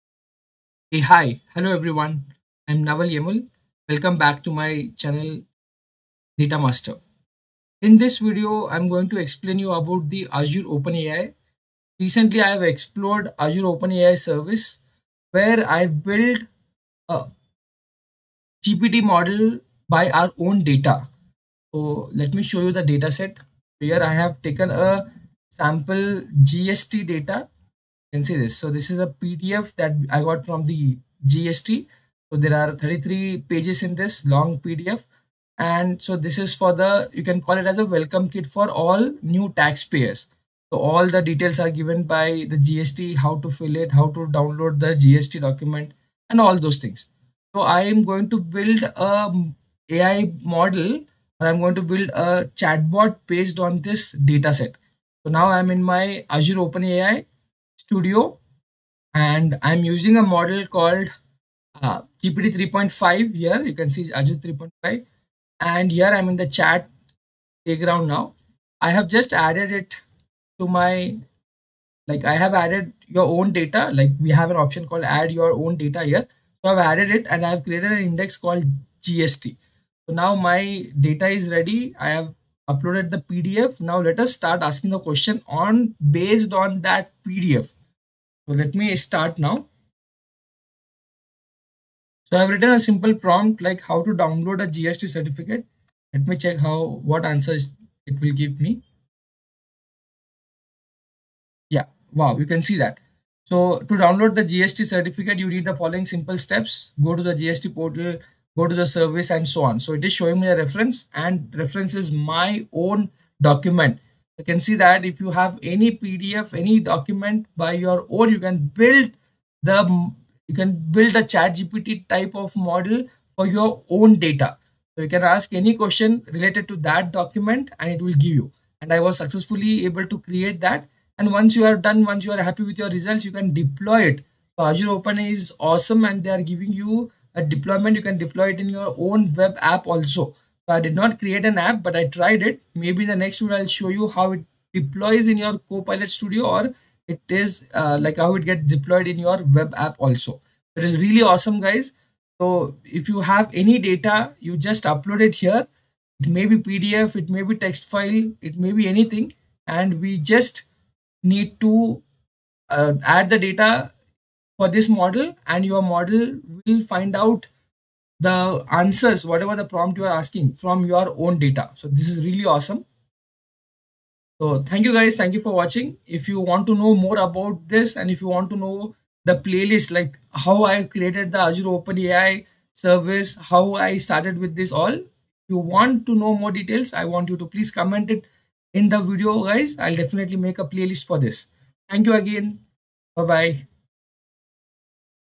Chat with your Data with Azure Open AI
In this video, we show you how to chat with your data using Azure OpenAI! Learn how to leverage the power of Azure OpenAI to interact with your data more efficiently, using natural language queries to extract valuable insights.

What you'll learn:
How to set up and integrate Azure OpenAI with your data sources.
Using natural language processing (NLP) to query your data and get real-time responses.
Best practices for building AI-powered chatbots that interact with data stored in Azure.
How Azure OpenAI can enhance data exploration, analysis, and decision-making processes.
This tutorial is perfect for anyone looking to integrate AI into their data workflows and take their data interaction to the next level.

Discover your potential in the Data Master Consulting Challenge. Click the Website link below to join now:

Welcome to [Your Channel Name]! In this exciting episode, we dive into the world of conversational AI powered by Azure Open AI. Imagine having natural and meaningful conversations with your data—well, now you can!

🌐 Azure Open AI Overview:
Azure Open AI brings cutting-edge natural language processing capabilities to the table, allowing you to interact with your data like never before. Whether you're a developer, data scientist, or just curious about the future of AI, this is the episode for you!

🔍 Key Features:
Conversational Insights: Uncover hidden patterns and valuable insights through a conversational interface.
Seamless Integration: Easily integrate Azure Open AI into your existing applications and workflows.
User-Friendly: No advanced coding skills required! Start chatting with your data effortlessly.

Developers keen on exploring the potential of conversational AI.
Data enthusiasts looking for innovative ways to interact with and analyze data.
Businesses seeking to enhance their decision-making processes with AI-driven insights.

📝 Agenda:
Introduction to Azure Open AI
Setting up your environment
Building a conversation with your data
Analyzing insights and patterns
Integrating with your applications
Future possibilities and advancements